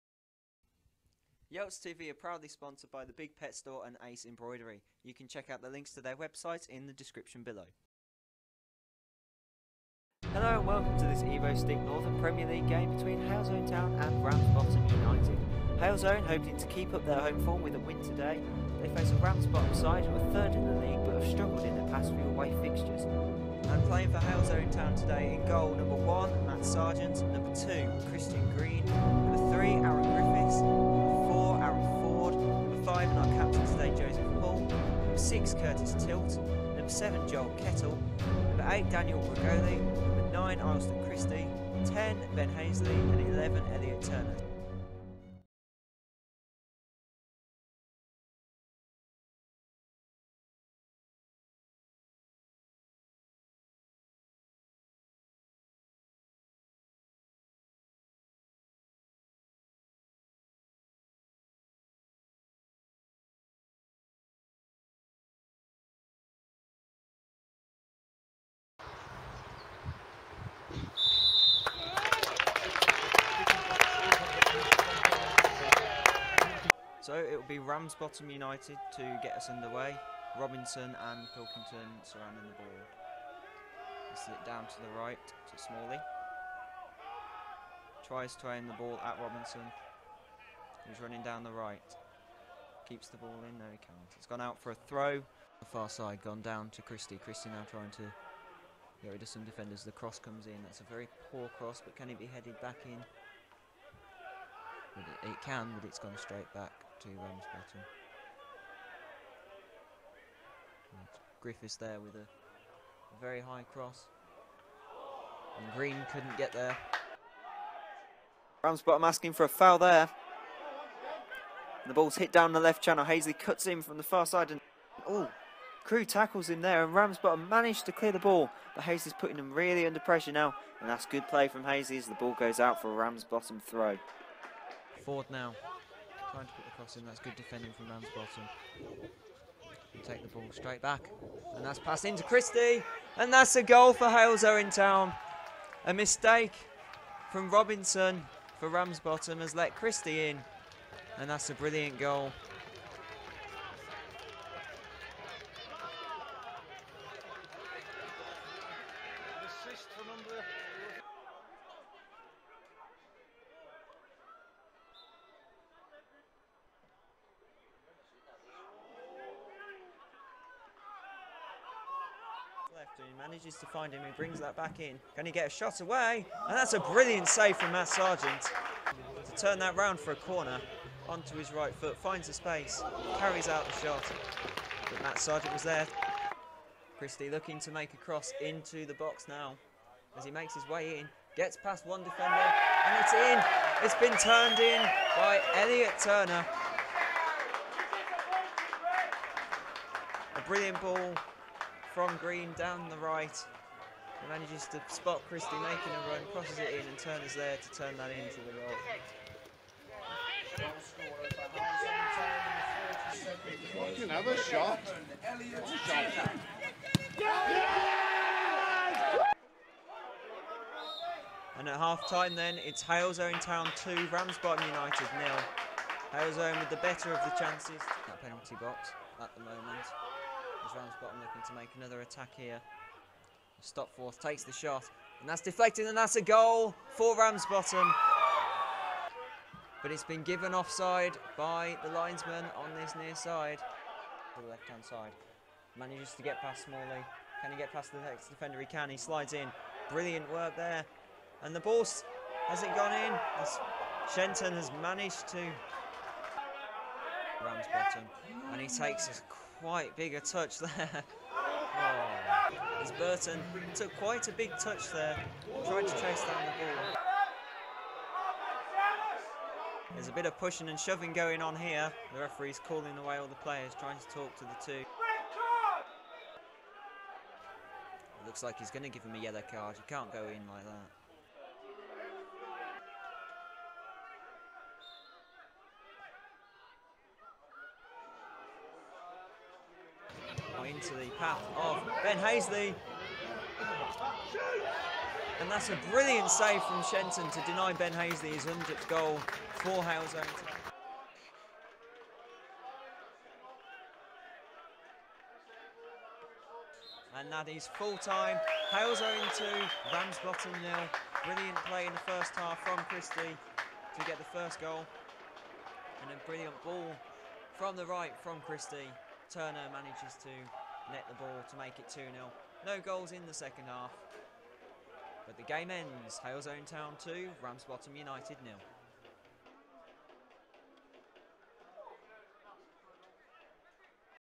Halesowen v Ramsbottom (Match Highlights)
Here are our match highlights from our game against Ramsbottom. Hoping to keep up the winning ways from the FA Trophy game vs Matlock, can John Hill's side gain three points in the league?

We are aware of slight audio issues, but have realised our problems so we can fix it from the next match we cover.
As always we appreciate all your support and patience with us as we try and perfect our videos for you all.